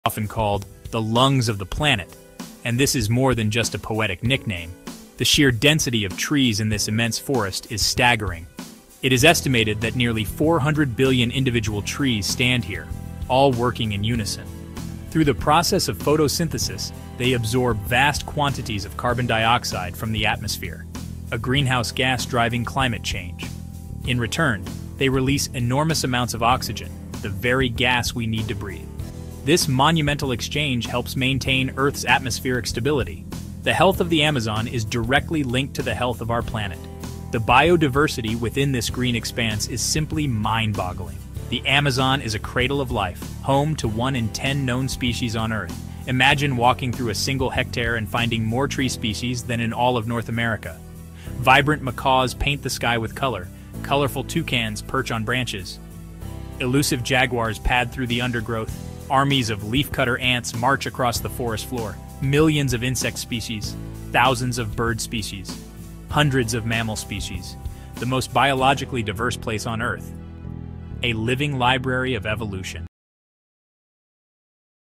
Why Amazon's OXYGEN PRODUCTION is Crucial for Our Planet
The Amazon rainforest's density of trees absorbs massive CO2, releasing vital oxygen. Home to 1 in 10 known species, its biodiversity is unmatched. The health of the Amazon is directly linked to the health of our planet.